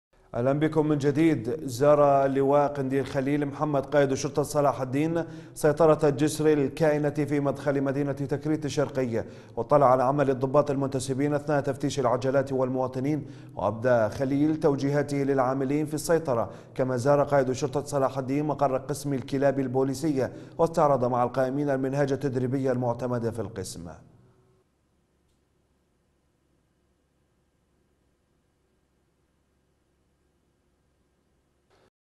قائد شرطة صلاح الدين الاطلاع ميدانيا على عمل ضباط ومنتسبي سيطرة الجسر
قائد شرطة صلاح الدين
الاطلاع ميدانيا على عمل ضباط ومنتسبي سيطرة الجسر

زار للواء قنديل خليل محمد قائد شرطة صلاح الدين سيطرة الجسر الكائنة في مدخل مدينة تكريت الشرقي , واطلع على عمل الضباط المنتسبين اثناء تفتيش العجلات والمواطنين , وابدى اللواء قنديل خليل توجيهاته للعاملين في السيطرة , كما زار قائد شرطة صلاح الدين مقر قسم الكلاب البوليسية واستعرض مع القائمين المنهاج التدريبي المعتمد في القسم .